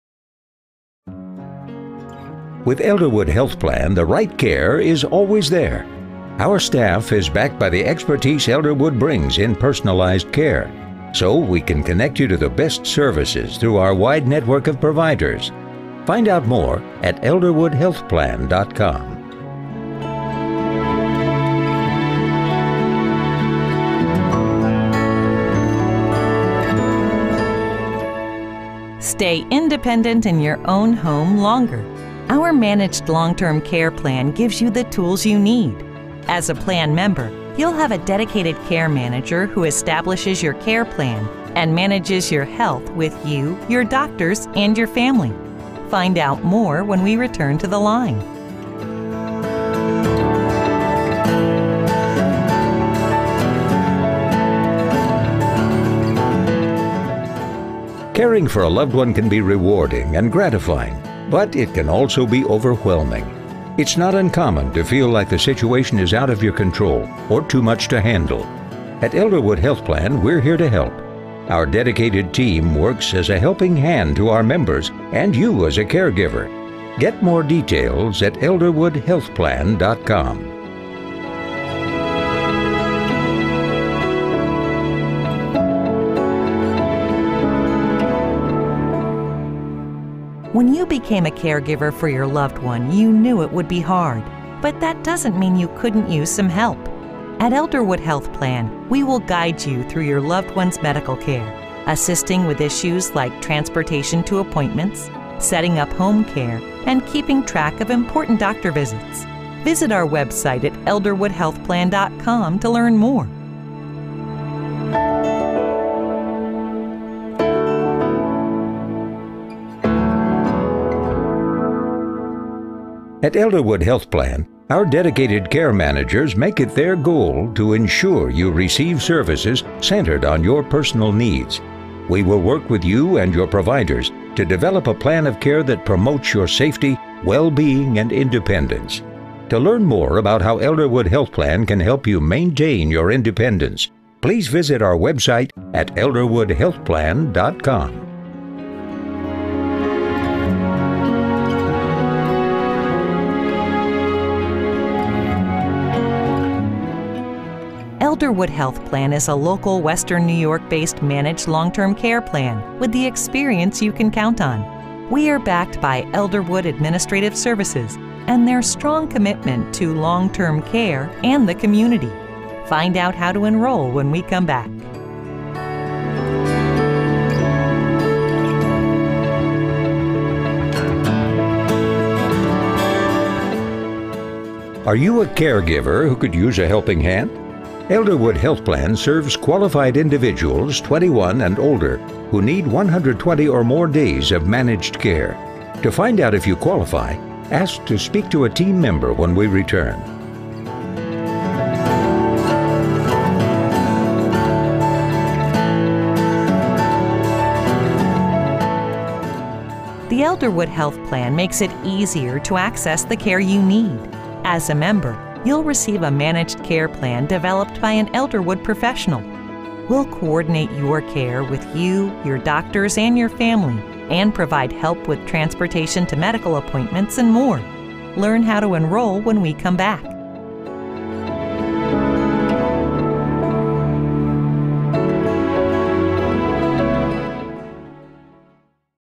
Elderwood Health Plan - On Hold Marketing from BusinessVoice
This On Hold Marketing production was created for Elderwood by BusinessVoice, one of the nation's leading On Hold Marketing agencies. It was named our On Hold Marketing of the Month for November 2015.